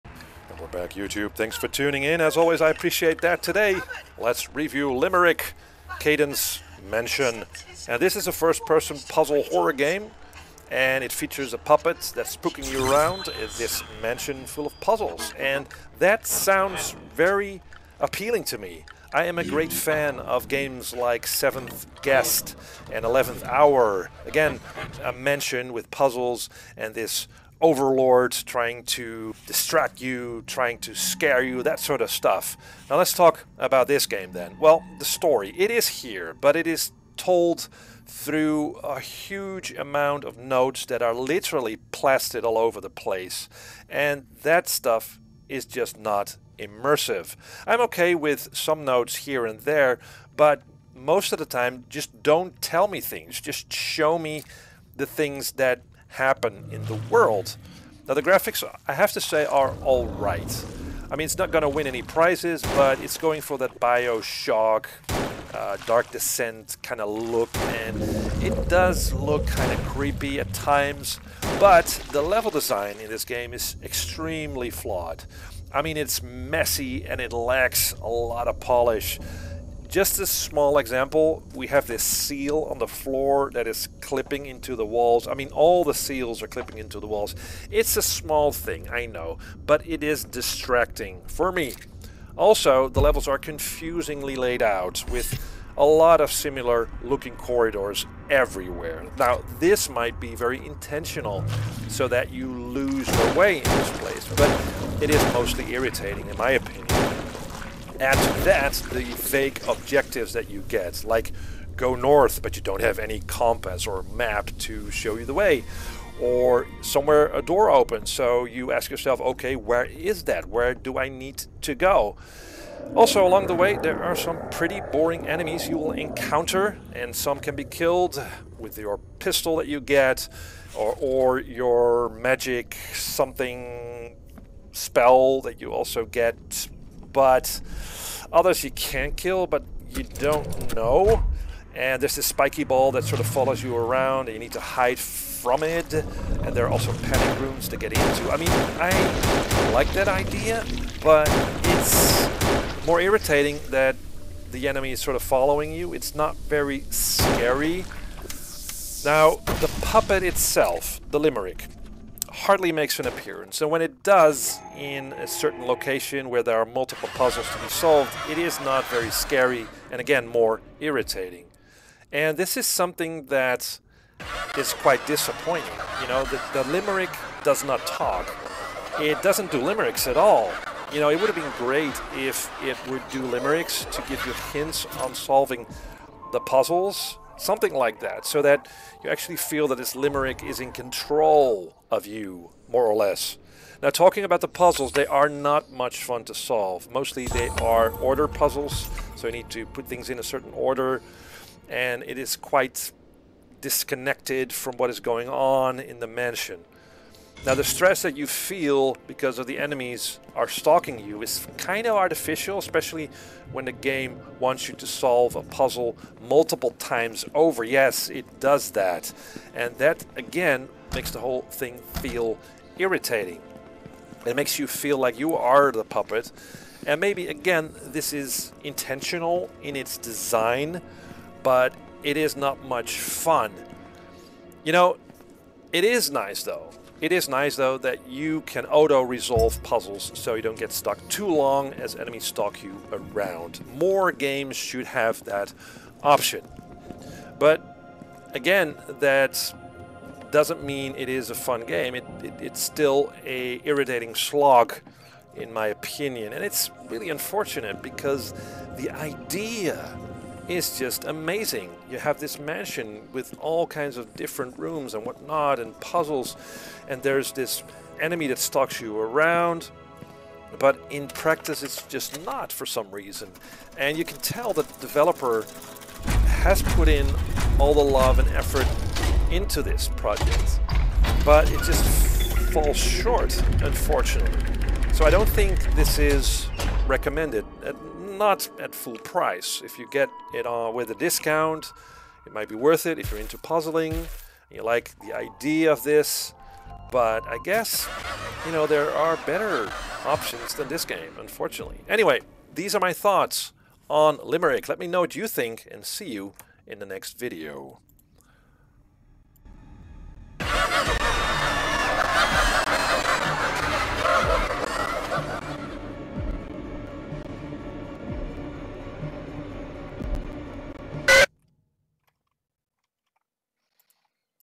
Limerick: Cadence Mansion Review
A flawed horror puzzle game.